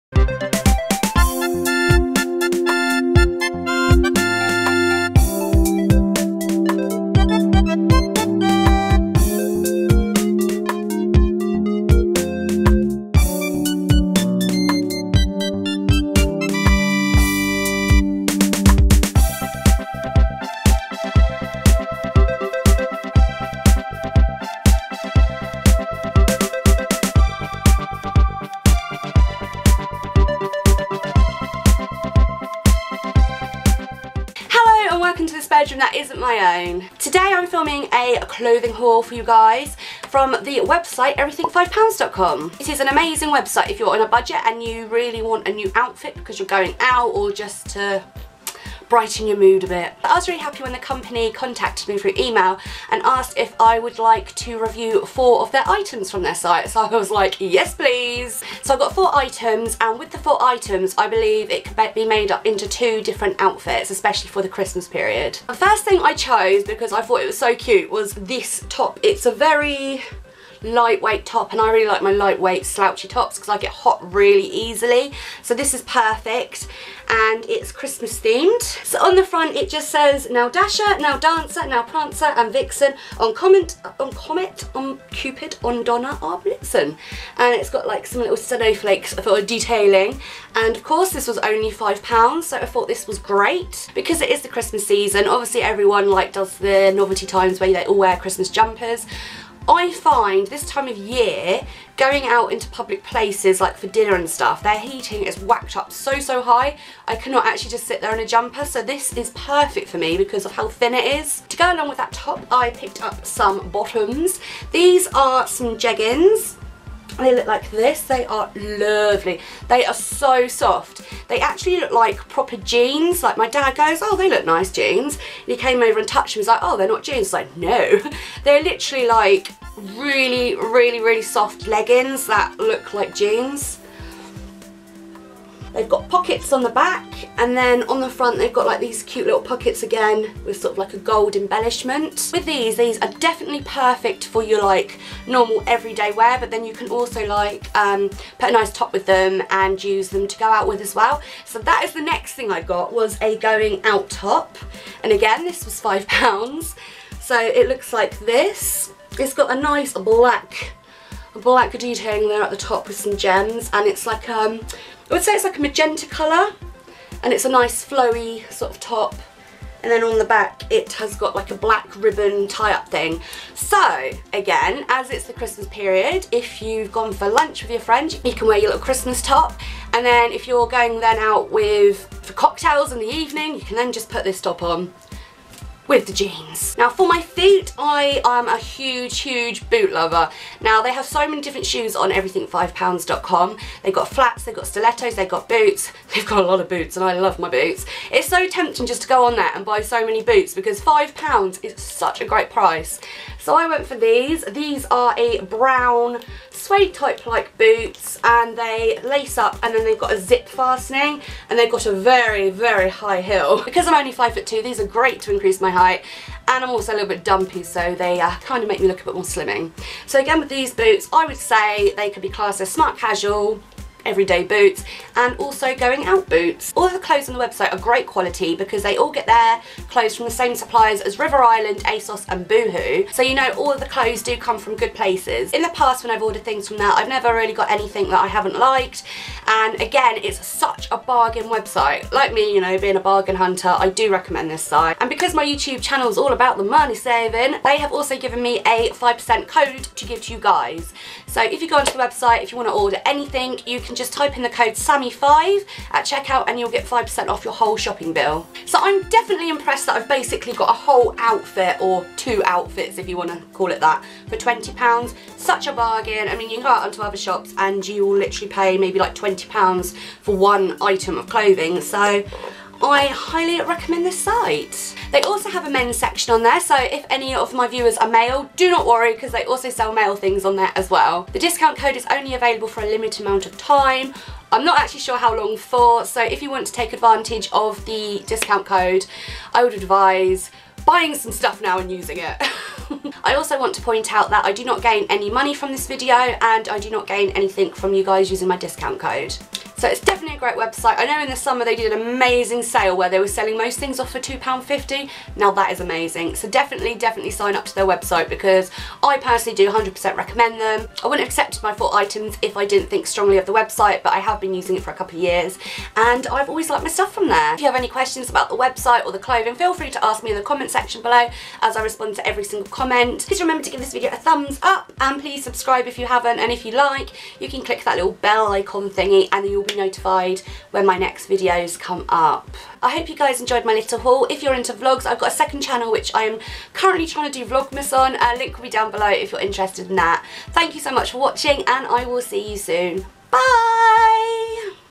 plus size | everything5pounds.com REVIEW/HAUL | yo sammy
Hello savvy friends! ive been using everything5pounds.com for a while now and love the bargains they have to offer!

AMAZON WISHLIST